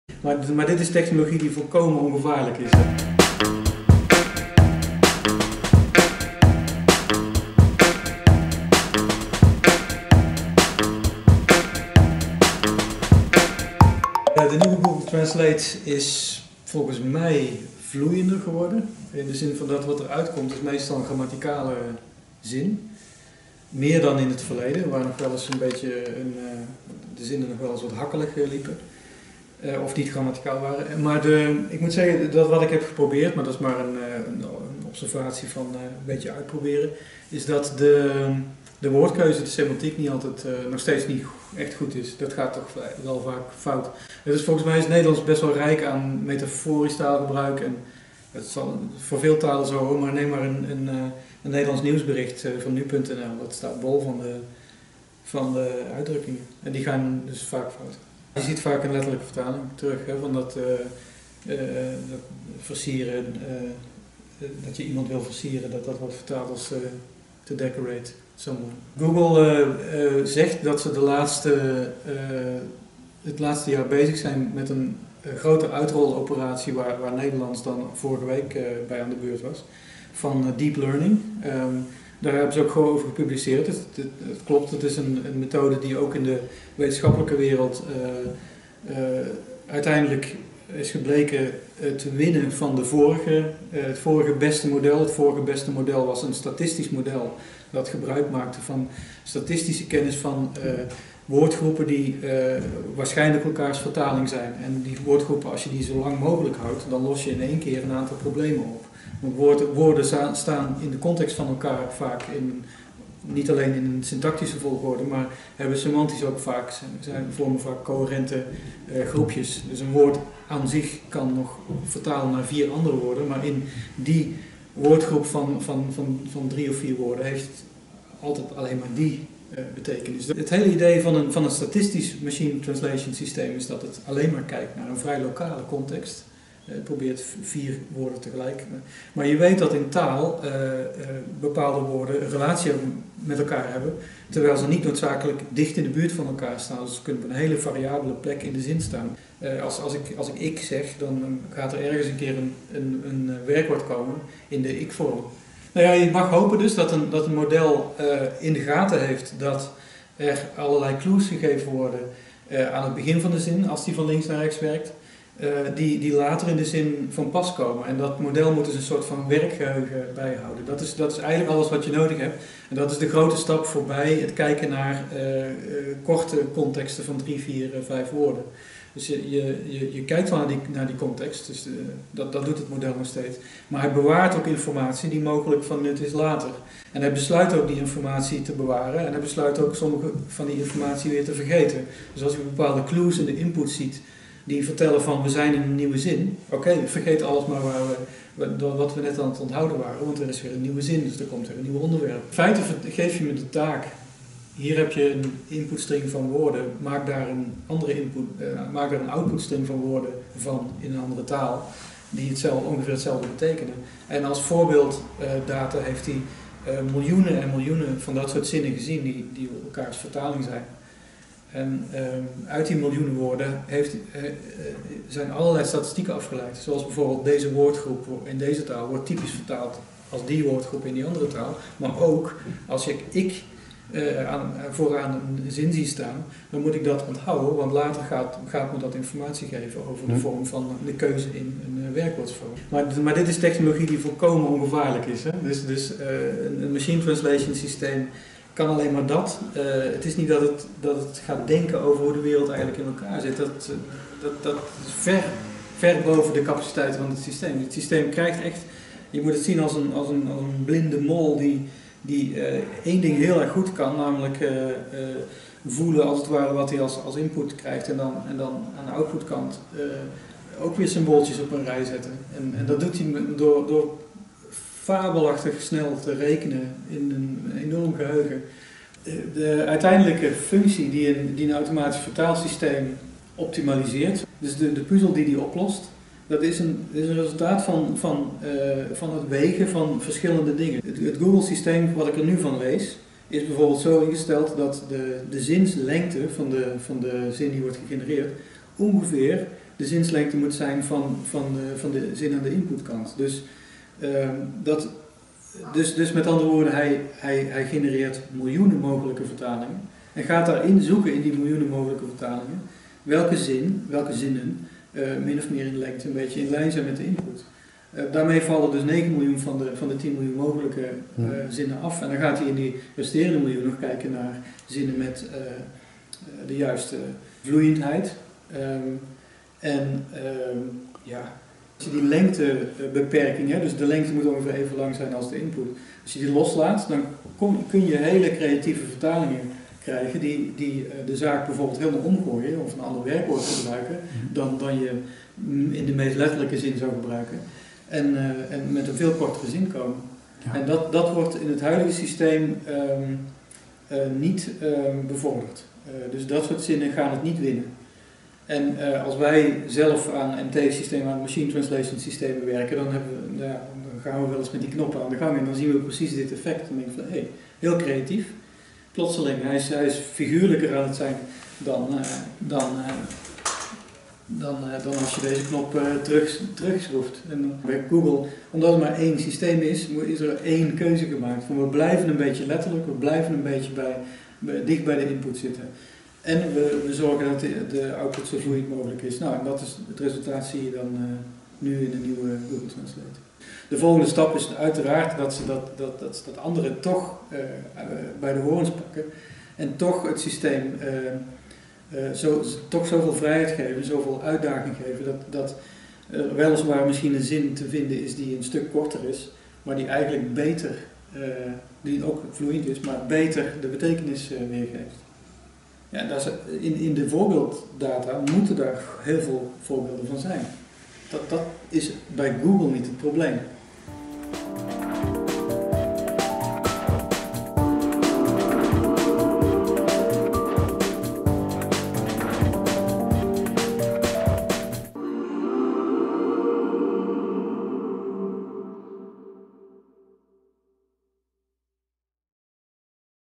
Hoe werkt Google Translate?
Vorige week kondigde Google Translate een nieuwe, verbeterde versie aan van de vertalingen van (onder andere) het Nederlands naar het Engels en omgekeerd. Hoe is de nieuwe versie verbeterd? En wat kan er nóg beter? Ik vroeg het aan Antal van den Bosch, de directeur van het Meertens Instituut en zelf als hoogleraar Taal- en Spraaktechnologie aan de Radboud Universiteit Nijmegen ook ervaren in het maken van vertaalcomputers.